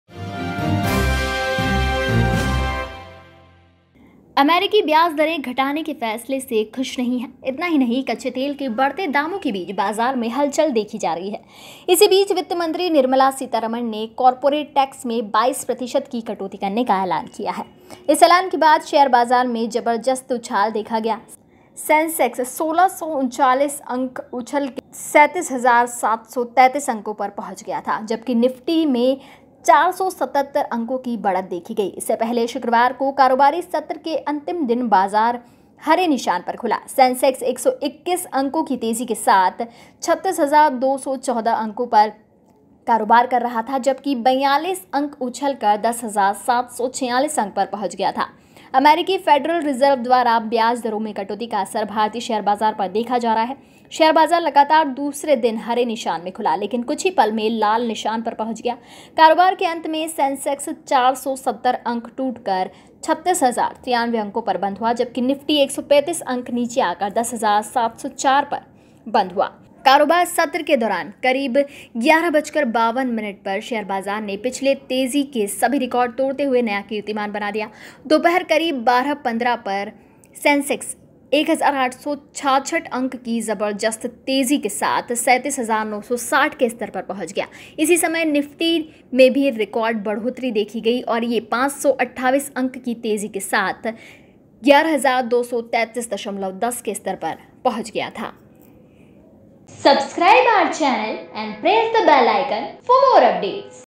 Share Market में 10 साल में सबसे बड़ा उछाल | Corporate Tax Reduced | Sensex Nifty Stock Market
कॉर्पोरेट टैक्स में छूट से शेयर बाजार में चमक
दिख रहा है अमेरिकी ब्याज दरों  में कटौती का असर